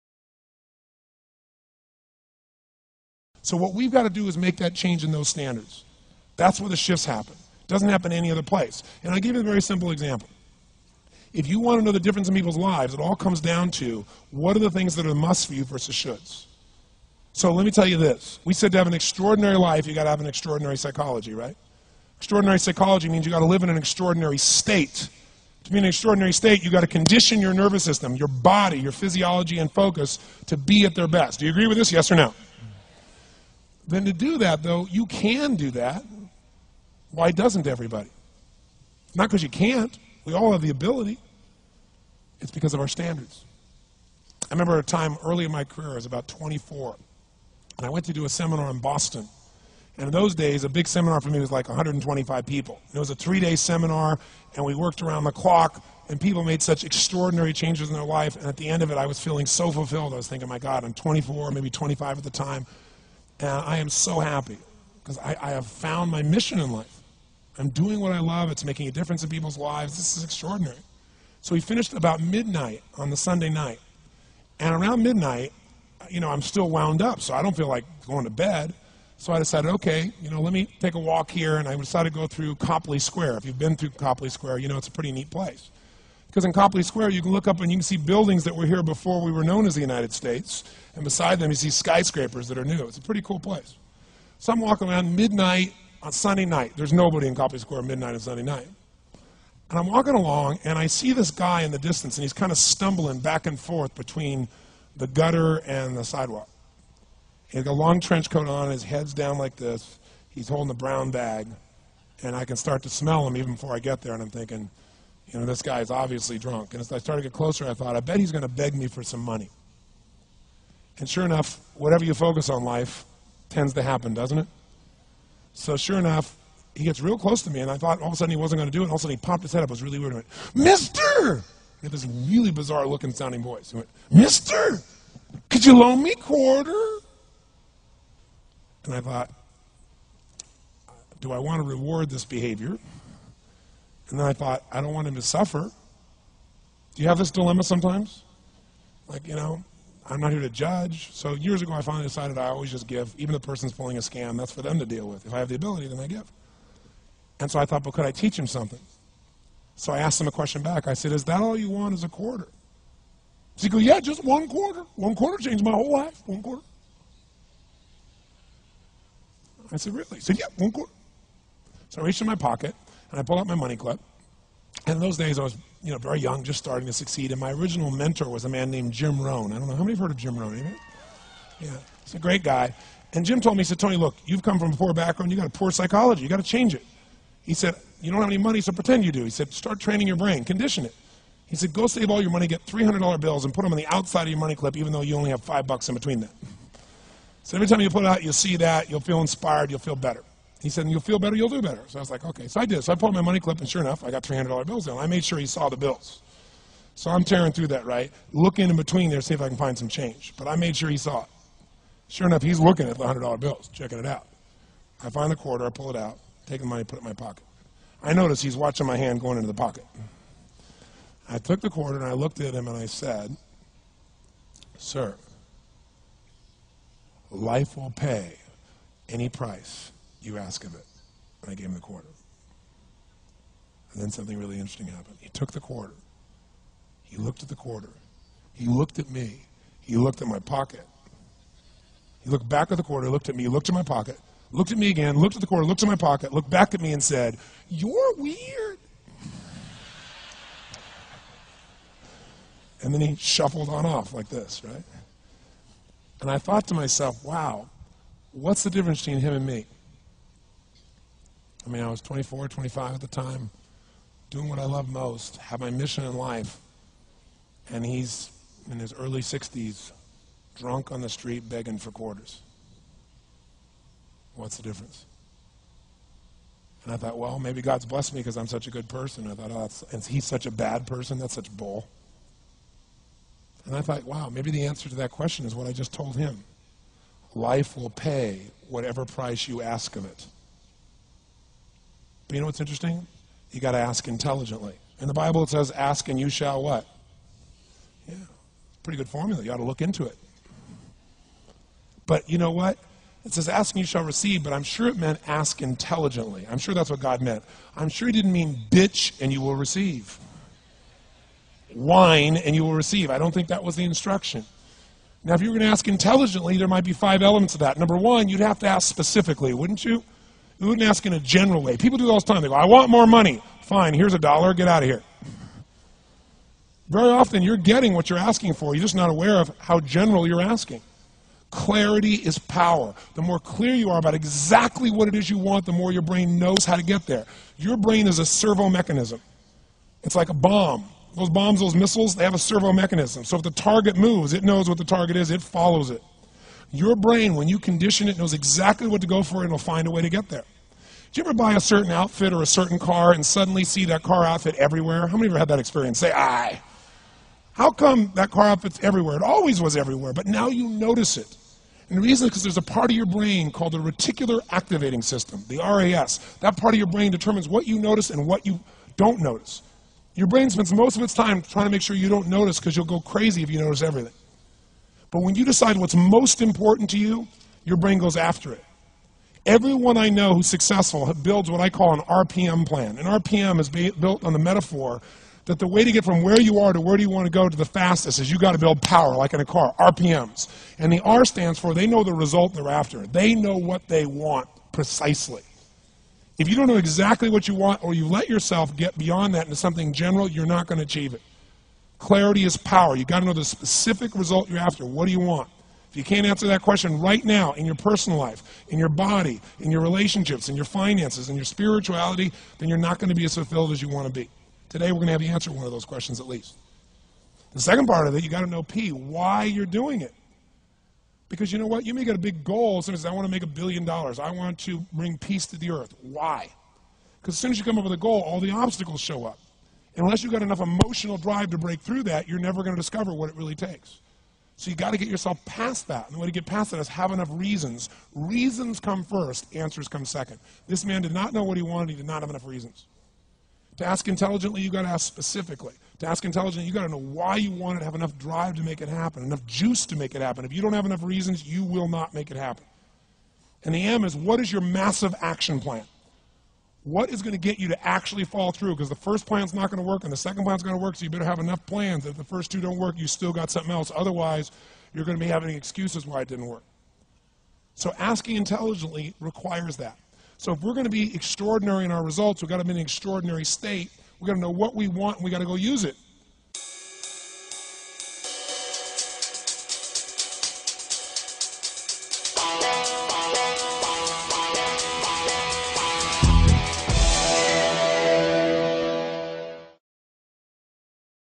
Tony Robbins | Shoulds vs. Musts
Noteworthy bullet points:
- Turn your 'shoulds' into 'musts.'
- Raise your standards.
- Ask intelligently.
- Ask specifically.
- Clarity is power.
- Reticular Activating System (RAS).
- Know the specific result you're after.
- Know WHY you're doing anything.
- Reasons come first; answers come second.
- Take MASSIVE ACTION.

"Life will pay any price you ask of it."
- Tony Robbins